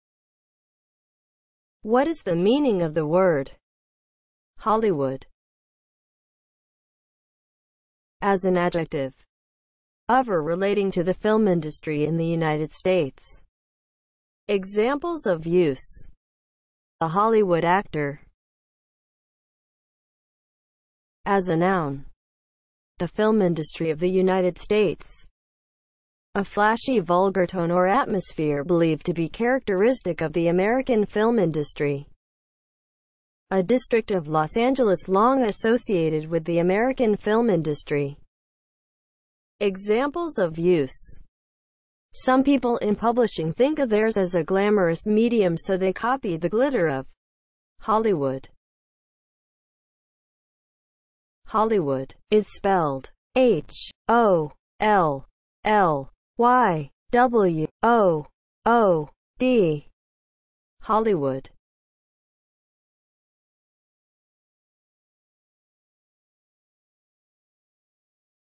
What is the meaning of the word HOLLYWOOD?
Definition, examples of use and spelling of the word HOLLYWOOD.

(00:00) Intro
(00:07) As an adjective
(00:20) As a noun
(00:50) Spelling

As an adjective (00:07):
• of or relating to the film industry in the United States

Examples:
• "a Hollywood actor"

As a noun (00:20):
• the film industry of the United States
• a flashy vulgar tone or atmosphere believed to be characteristic of the American film
  industry
• a district of Los Angeles long associated with the American film industry

Examples:
• "some people in publishing think of theirs as a glamorous medium so they copy the glitter of Hollywood"